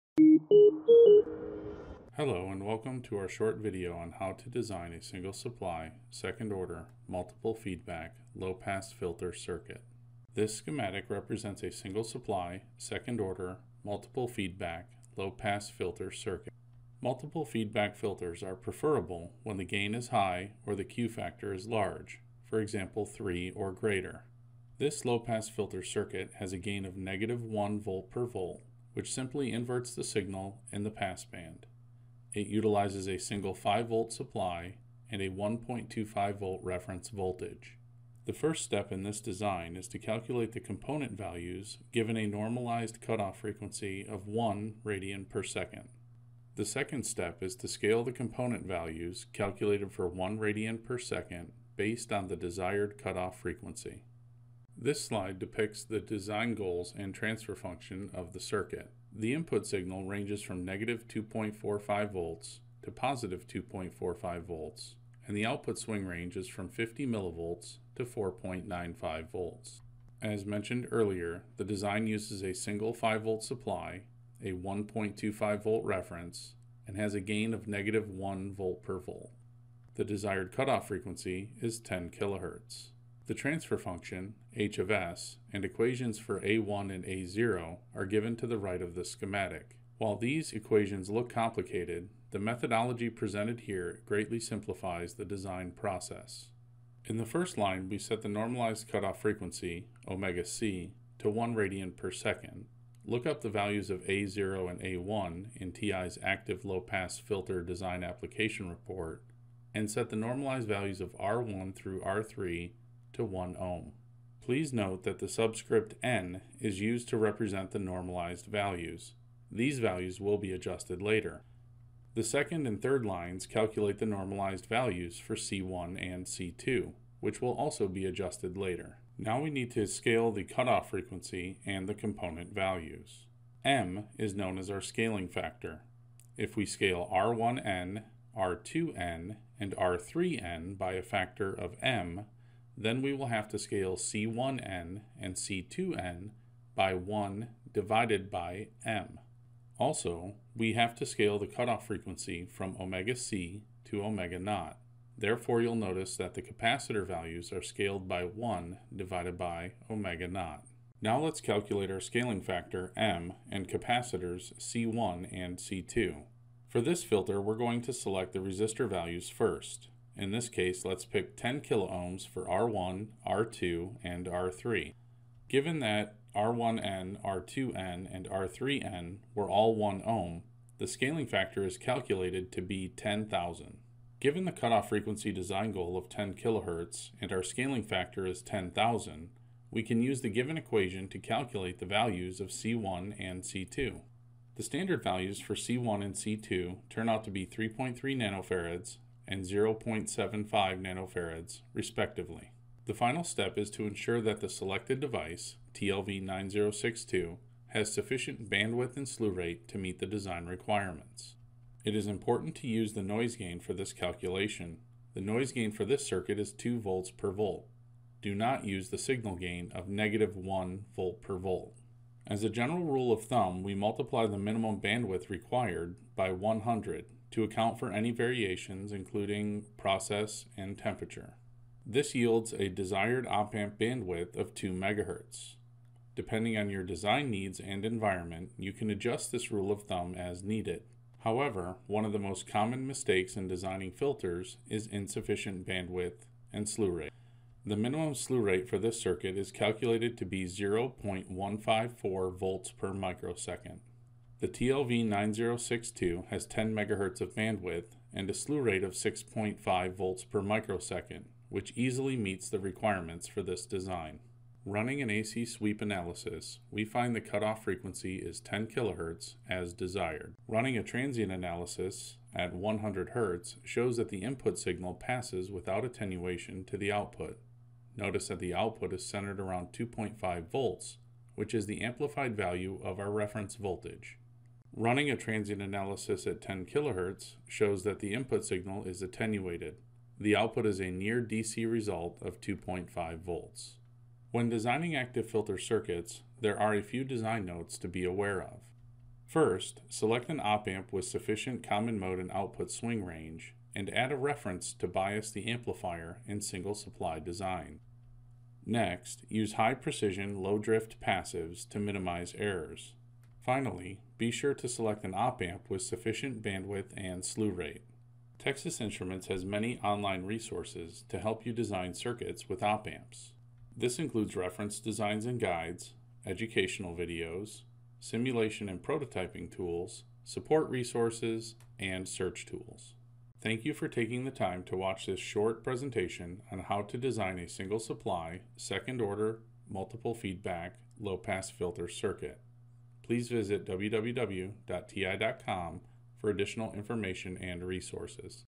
Single-supply, 2nd-order, multiple feedback low-pass filter circuit
Order samples and learn more about the op amp used in this video

The multiple-feedback (MFB) low-pass filter (LP filter) is a second-order
active filter. Vref provides a DC offset to accommodate for single-supply
applications. This LP filter inverts the signal (Gain = –1 V/V) for
frequencies in the pass band. An MFB filter is preferable when the gain is
high or when the Q-factor is large (for example, 3 or greater).